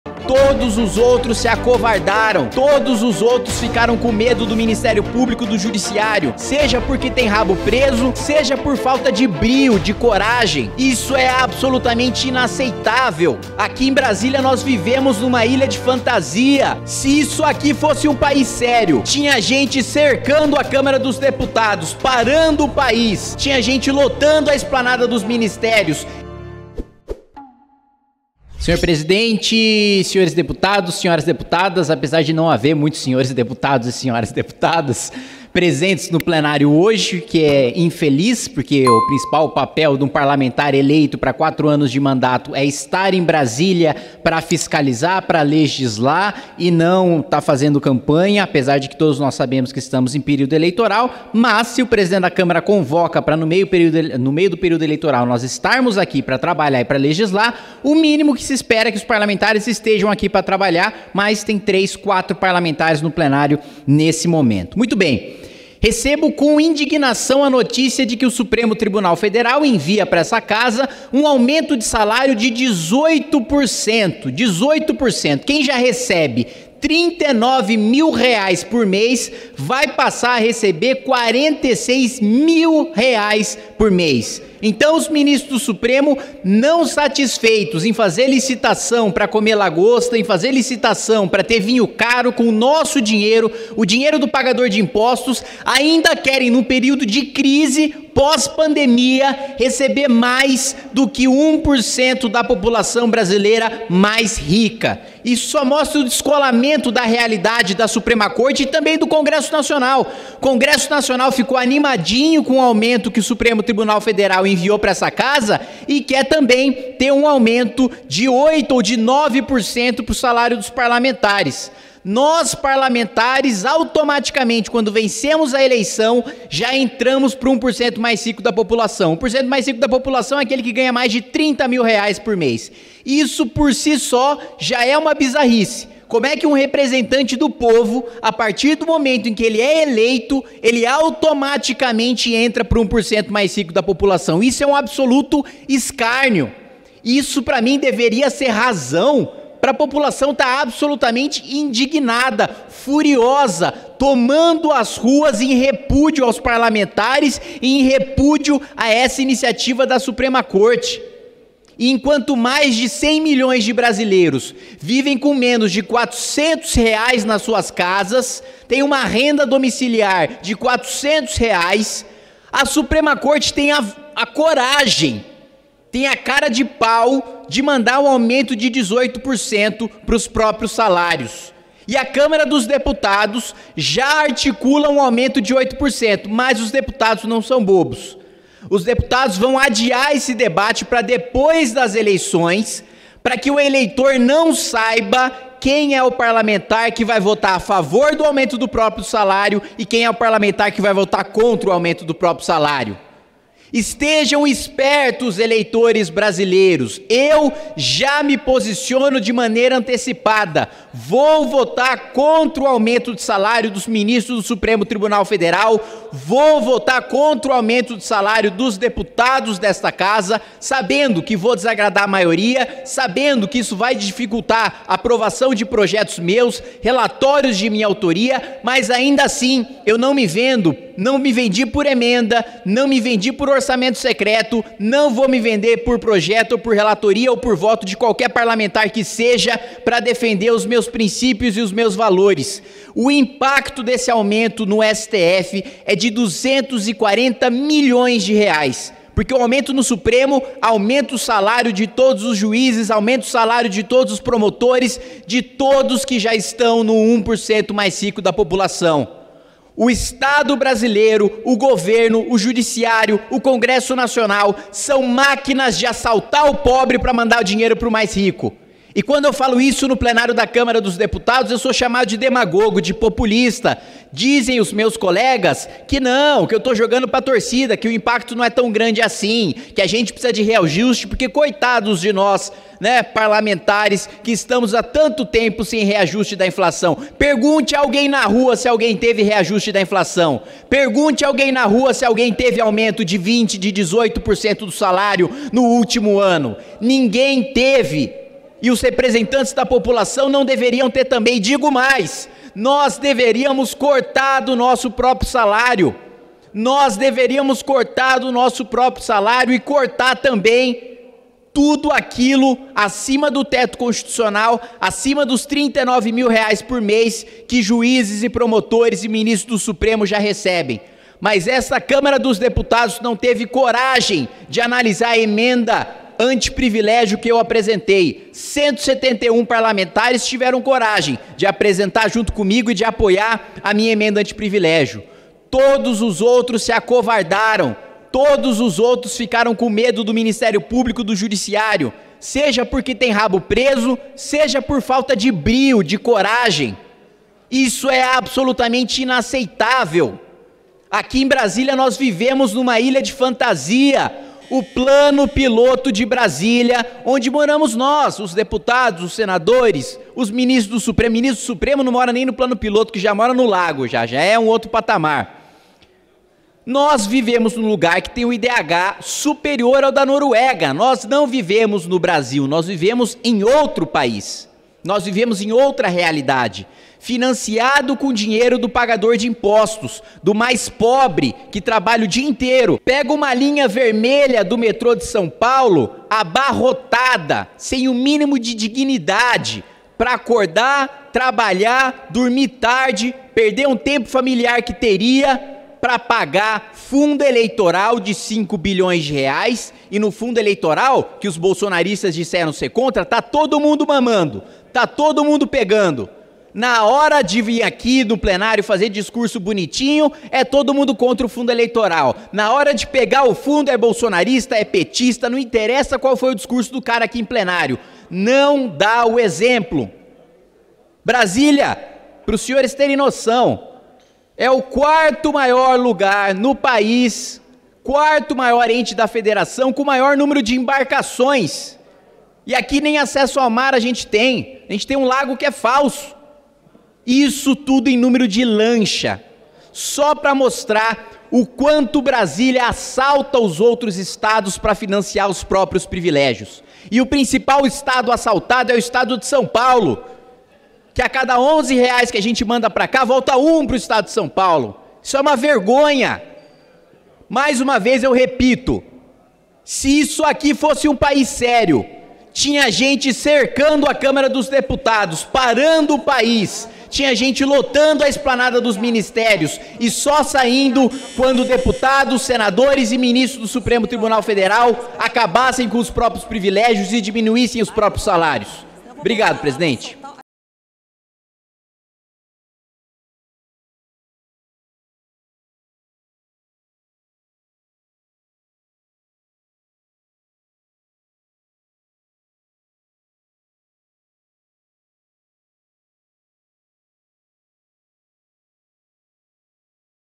VOU BARRAR aumento do STF e do Congresso!
É inacreditável: querem dar aumento salarial para os ministros do STF. Vão obrigar que você pague mais impostos para bancar o salário da elite do funcionalismo público que já ganha muito. Um verdadeiro escárnio.